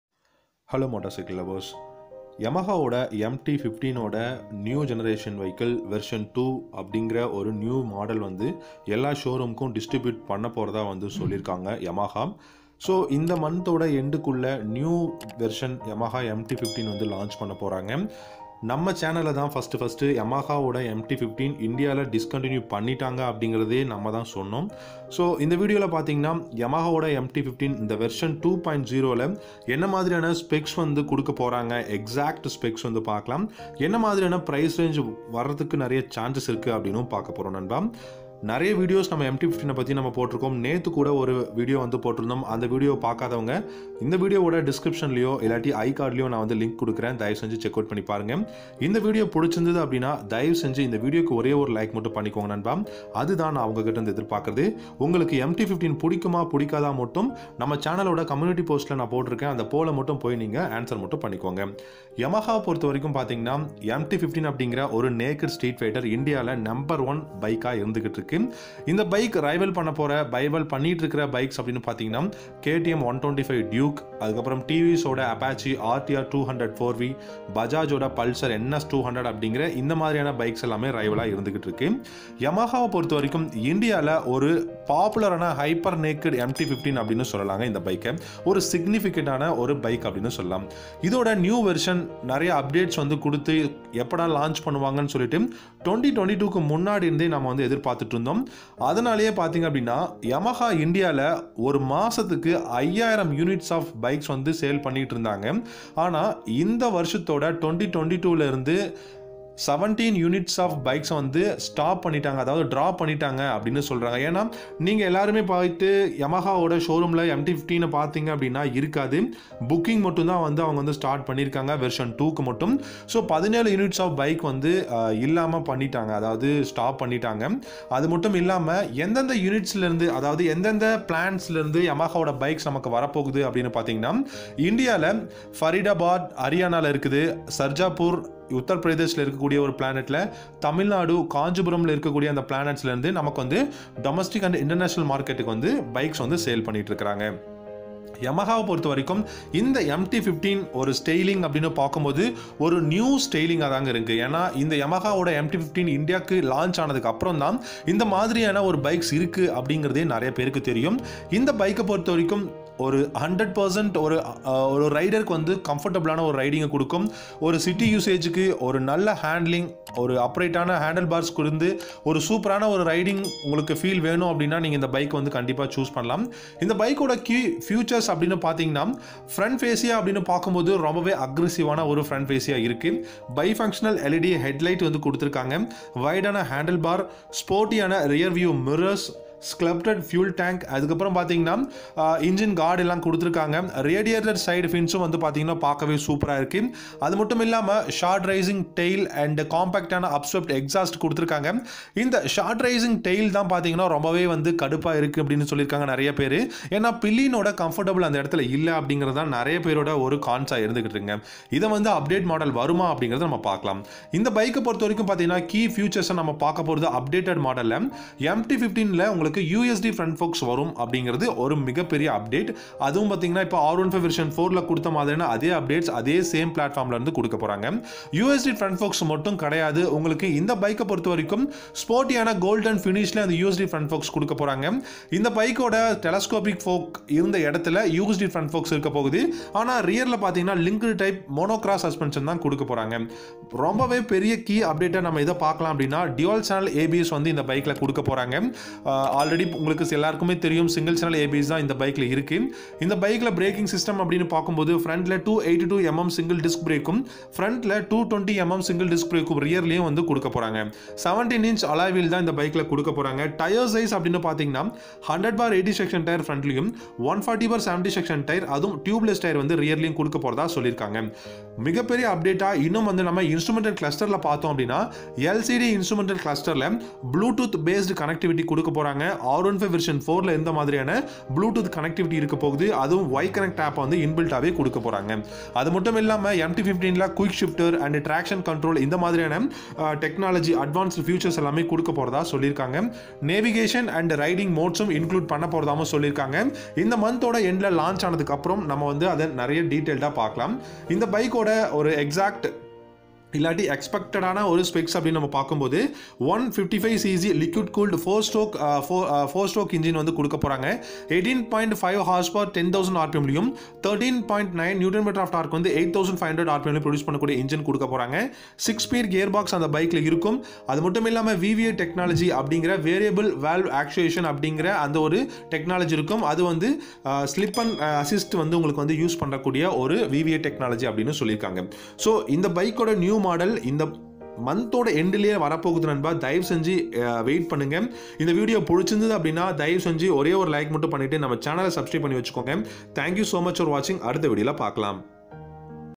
Finally 2022 Yamaha MT-15 Showroom வந்தாச்சு | New Updates | epicriderjayz
Hey Guy's this video is all about upcoming yamaha MT-15 New Model Launch In India 2022..

🎬Popular Video's Link 🔗 Here Please Check ✔️

l